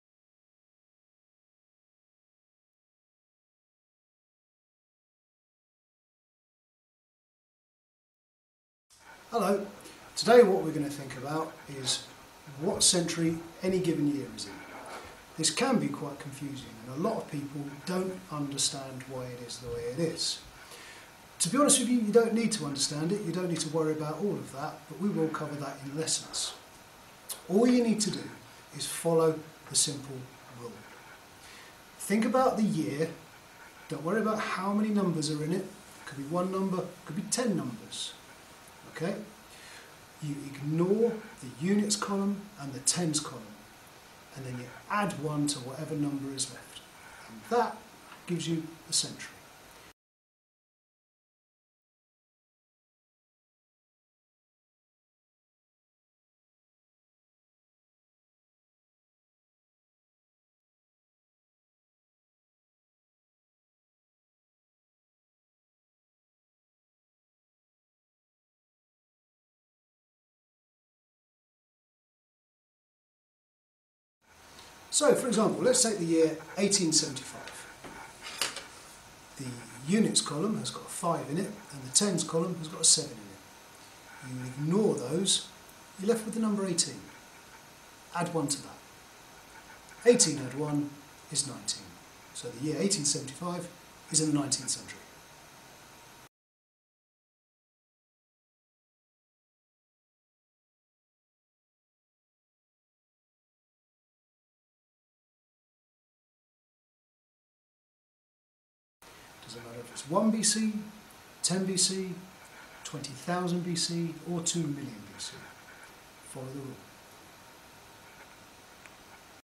How do you know what century you're in?
Mr Flynn guides you through the key skill of knowing what century a particular year is in.